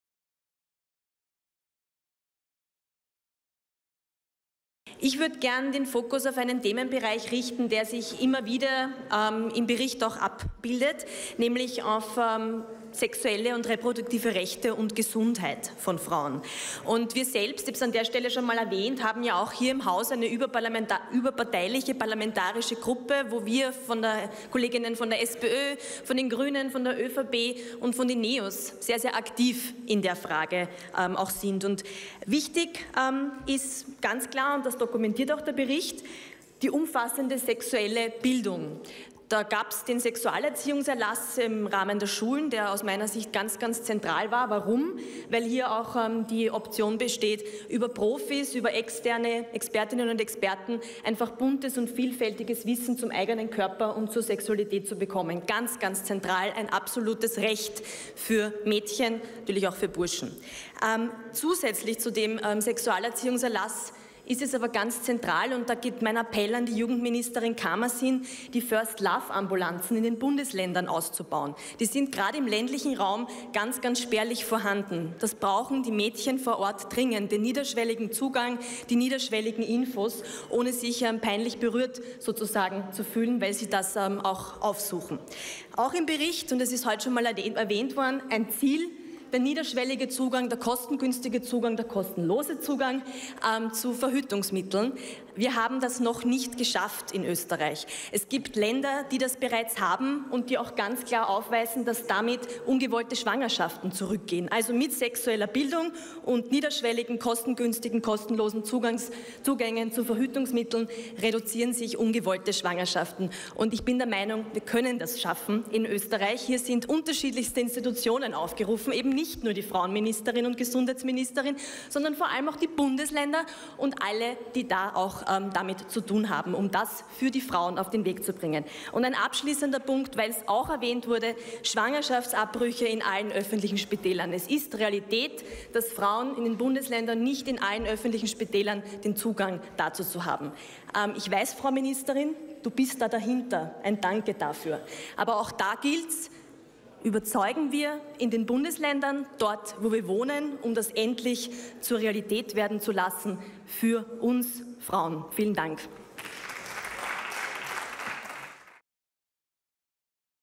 Es geht um sexuelle und reproduktive Rechte und Gesundheit! - Katharina Kucharowits
Erfreut zeigen sich die SPÖ-Abgeordneten Katharina Kucharowits und Karin Greiner über den Aktionsplan Frauengesundheit, der am Mittwoch im Nationalrat beschlossen wurde. Besonders wichtig für die Gesundheit von Frauen seien die sexuellen und reproduktiven Rechte sowie, dass Frauen über ein „umfassendes Wissen über ihren Körper und ihre Sexualität verfügen“, betonte Kucharowits. Die negativen Auswirkungen von Armut und Gewalt auf die psychische und physische Gesundheit von Frauen hob Greiner hervor: „Der Aktionsplan zielt auch auf diese Ursachen und Risikofaktoren ab.“

Um das grundlegende Recht von Mädchen und Frauen, über ihren Körper und ihre Rechte Bescheid zu wissen, zu stärken, habe sich eine überparteiliche parlamentarische Gruppe gegründet. „Der Sexualerziehungserlass war ganz zentral, um sexuelle Bildung als Voraussetzung für selbstbestimmte Sexualität zu fördern“. Nachholbedarf sieht sie hingegen bei den First-Love-Ambulanzen im ländlichen Raum. „Hier gilt mein Appell an Bundesministerin Karmasin, ein größeres Angebot für den niederschwelligen Zugang zur Sexualberatung zur Verfügung zu stellen.“

Für die SPÖ-Abgeordnete Karin Greiner liegt der Fokus auf den sozioökonomischen Einflussfaktoren auf die psychische und physische Gesundheit von Frauen. „Wollen wir die Gesundheit von Frauen stärken, müssen wir die soziale Ungleichheit in den Blick nehmen und die Einkommensunterschiede abbauen“, so Greiner. Außerdem sei jede fünfte Frau im Laufe ihres Lebens von Gewalt betroffen – Gewaltprävention müsse daher in  der Ausbildung von ÄrztInnen und PflegerInnen eine wichtige Rolle spielen.